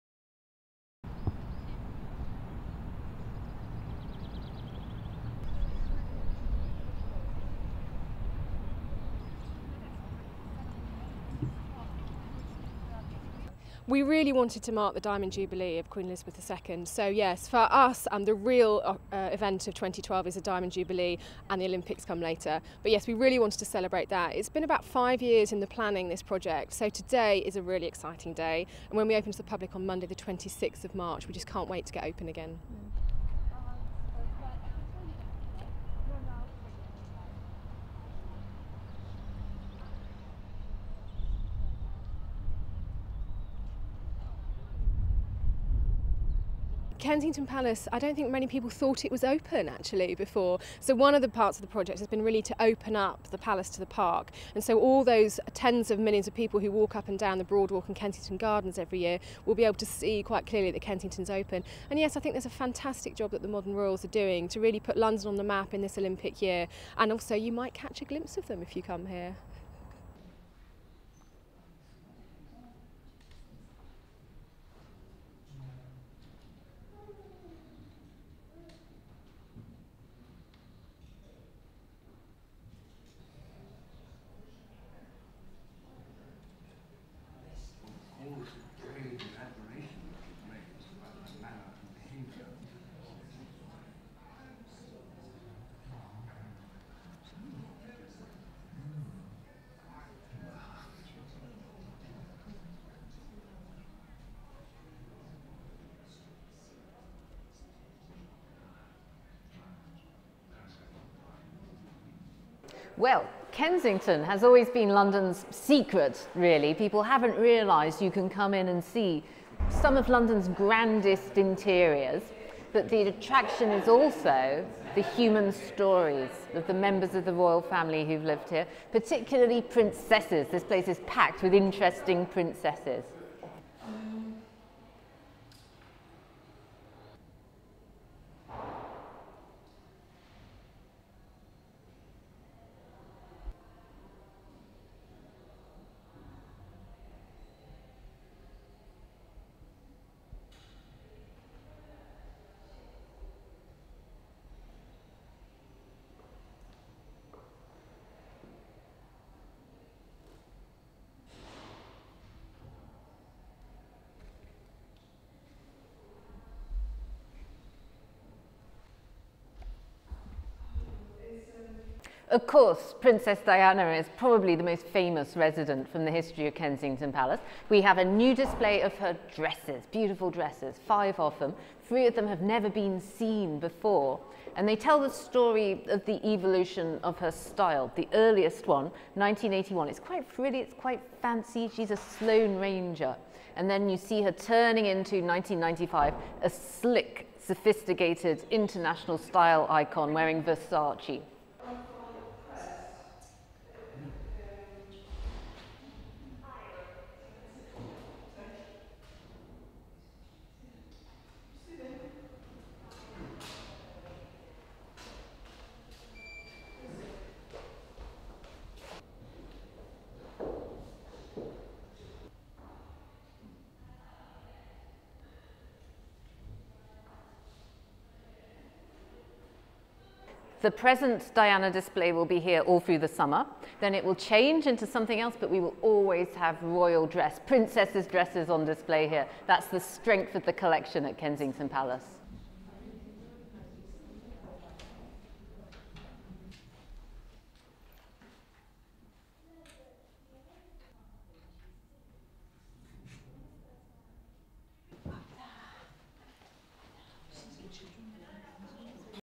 Princess Diana's dresses go on display at Kensington Palace
(20 Mar 2012) A 12 MILLION TRANSFORMATION FOR KENSINGTON PALACE
One of the Uk's lesser-known Royal abodes, Kensington Palace, is about to reoopen its doors to the public.
The palace has undergone a �12 million renovation to tie in with the Queen's Diamond Jubilee this year.
"For us the real event of 2012 is the Diamond Jubilee and the Olympics come later," explained Natasha Woollard, Visitor Experience Manager at Kensington Palace on Tuesday (20 MAR. 2012).
"But yes we really wanted to celebrate that. It's been about five years in the planning this project so today is a really exciting day and when we open to the public on Monday 26th March we just can't wait to get open again."
"Kensington Palace, I don't think many people thought it was open actually before so one of the parts of the project has been to really open up the palace to the park," Woollard continued.
"So all those tens of millions of people who walk up and down the broadwalk and Kensington Gardens every year will be able to see quite clearly that Kensington is open. And yes I think it's a fantastic job that the modern royals are doing to really put London on the map in this Olympic year and also you might catch a glimpse of them if you come here."
The renovation work has included extensive landscaping to the palace gardens and a ten-room "Victoria Revealed" exhibition, which uses over 300 items, coupled with quotes from private journals, to give insight into the life of Queen Victoria.
A temporary exhibition, "Diana: Glimpses of a modern princess," also showcases five of Diana's dresses.
They include the black silk taffeta Emanuel gown worn on the Princess' first official engagement with the Prince of Wales (to Goldsmiths' Hall, London, for a fundraising event in aid of the Royal Opera House), the ivory silk crepe Catherine Walker number which she wore for her iconic shoot by Mario Testino, and a black ribbed shift by Gianna Versace, worn to the London premiere of "Apollo 13."
"Of course Princess Diana is probably the most famous resident from the history of Kensington Palace," Lucy Worsley, Chief Curator at Historic Royal Palaces commented.
"We have a new display of her dresses, beautiful dresses, five of them. Three of them have never been seen before and they tell the story of the evolution of her style. The earliest one, 1981, it's quite frilly it's quite fancy, she's a sloane ranger, and then you see her turning into, 1995, a slick sophisticated international style icon wearing Versace."
"Diana: Glimpses of a modern princess" will be open to the public from 26th March to 2nd September 2012.